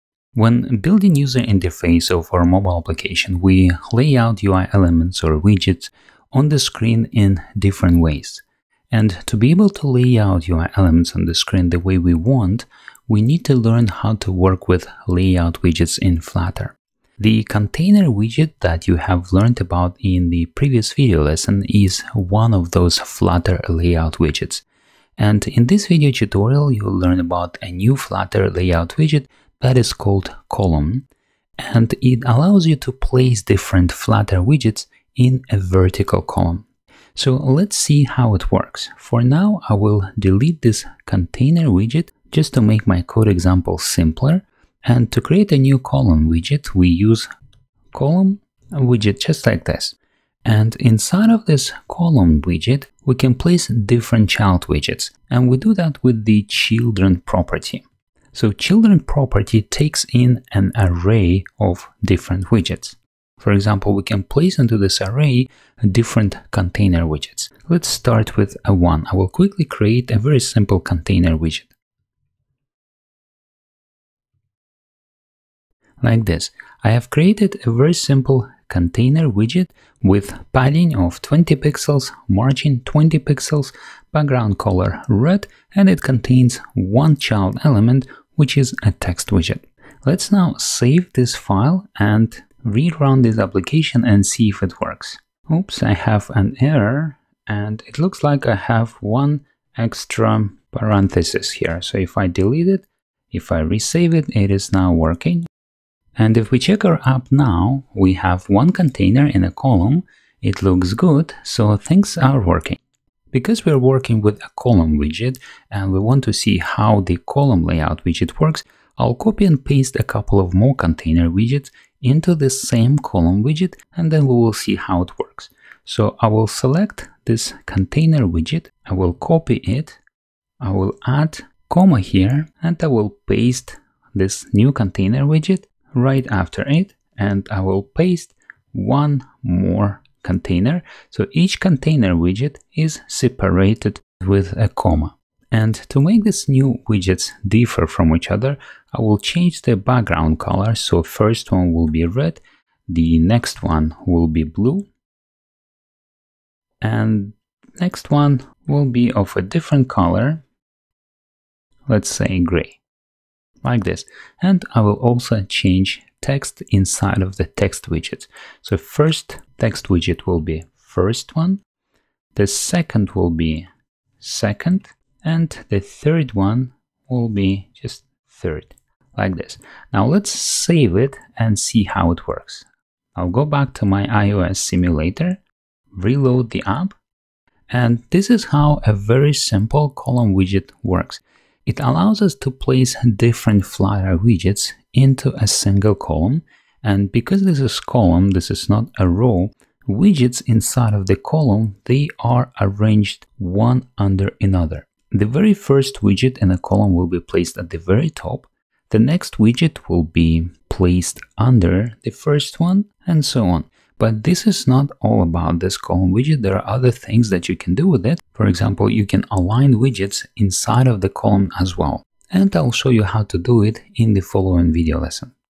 Flutter Column Layout Widget Tutorial.
In this video lesson, you will learn how to place different Flutter widgets in a column. You will learn how to create a very simple Flutter layout widget called "Column" and then place a few more container widgets into a single column.